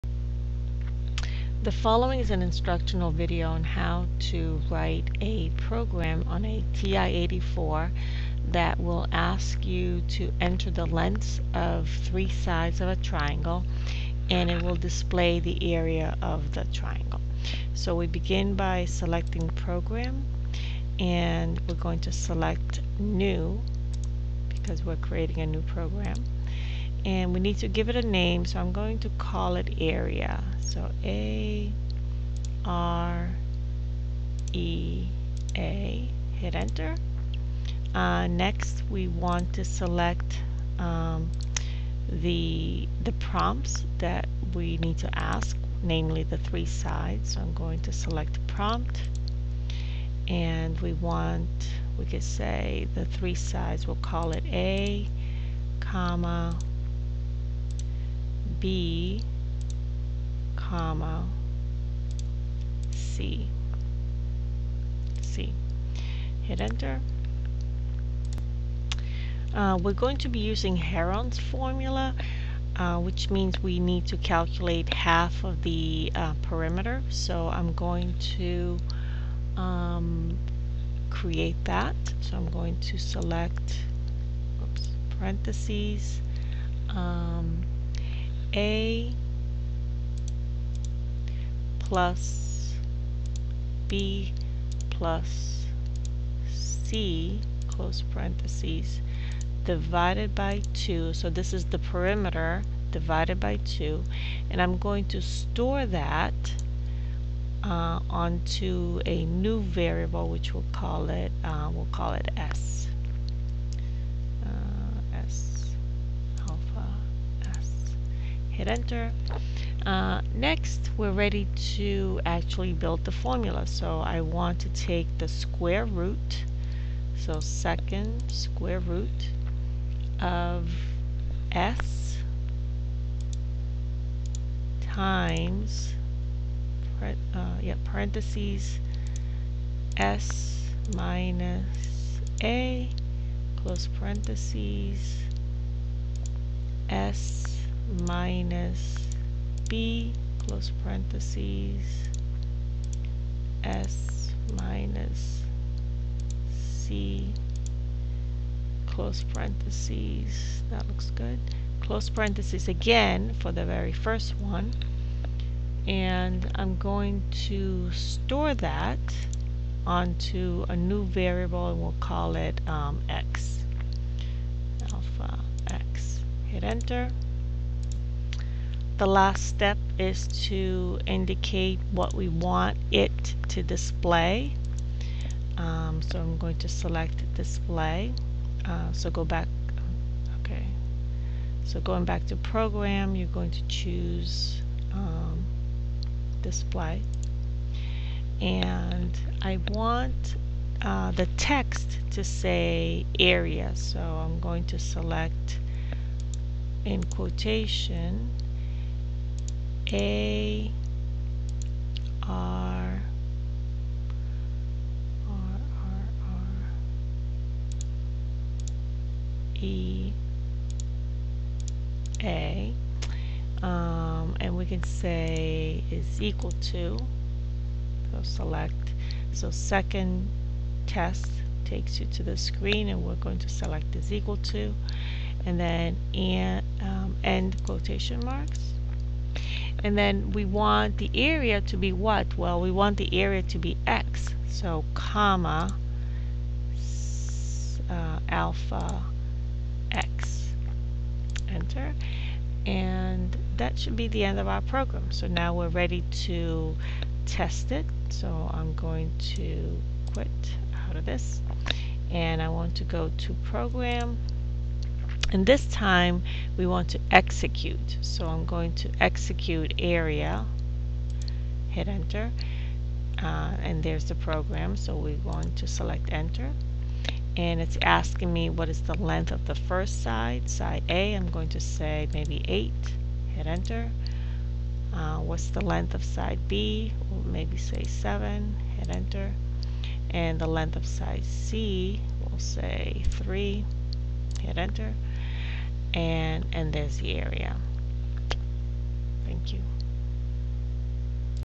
TI-84 area of triangle program
TI-84 program to calculate the area of a triangle.